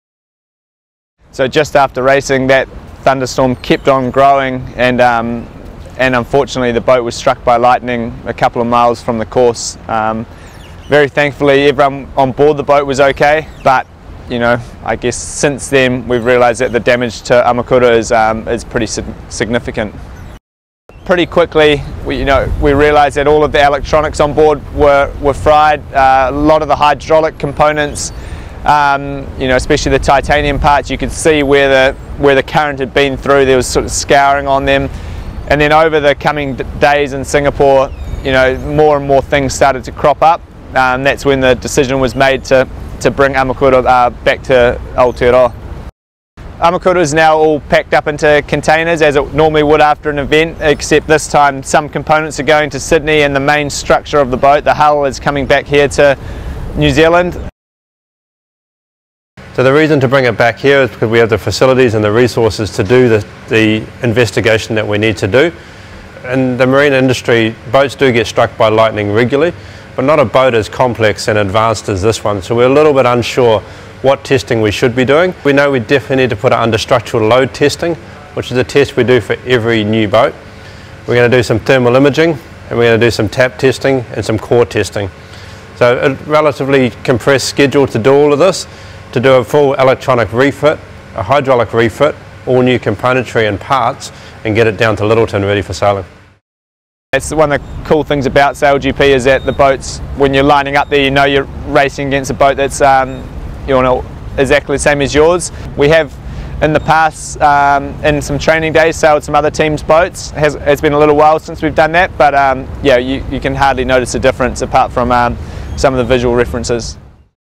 Kiwi F50 repair undertaken by SailGP Technologies
Kiwi F50 repair undertaken by SailGP Technologies after damage by lightning strike in Singapore